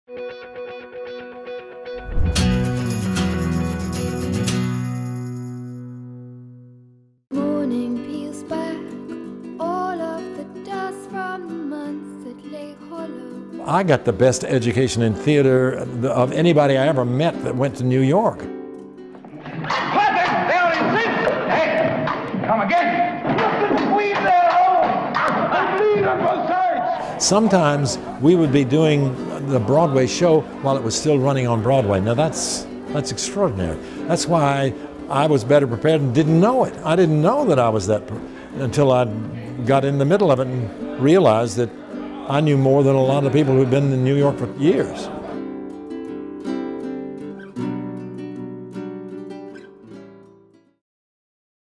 Alumni Memories: John Cullum
During commencement week, notable alumni are sharing their favorite memories from their time on Rocky Top.

John Cullum, two-time tony award winner and known for his role in the TV show Northern Exposure, speaks about his time at the Clarence Brown Theatre.